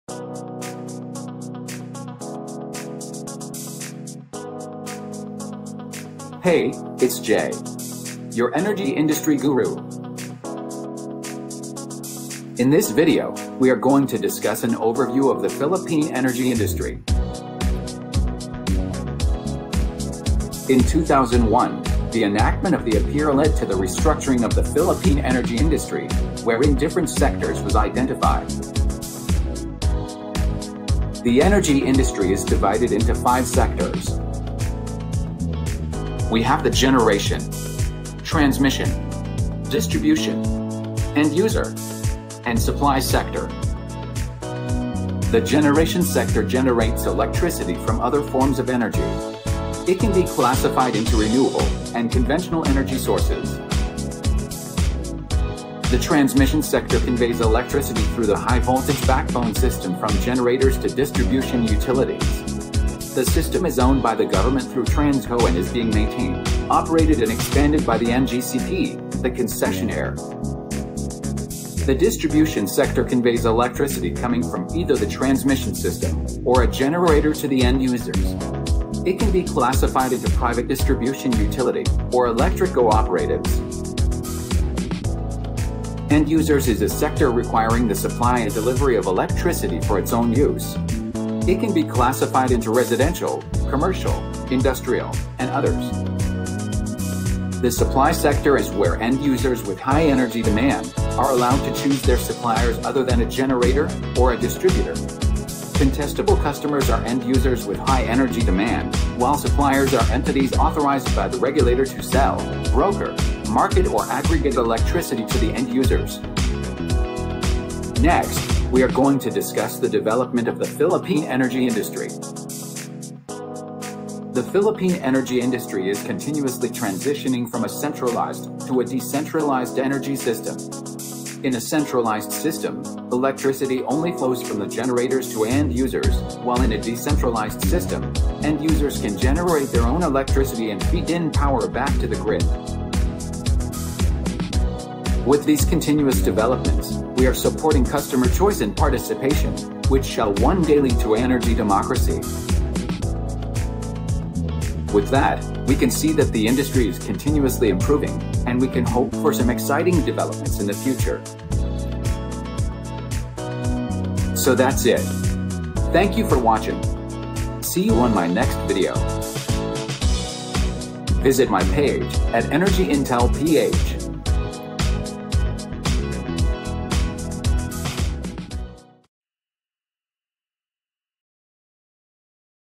Philippine Electric Power Industry
An overview of the Philippine Electric Power Industry wherein its Sectors and Developments was discussed.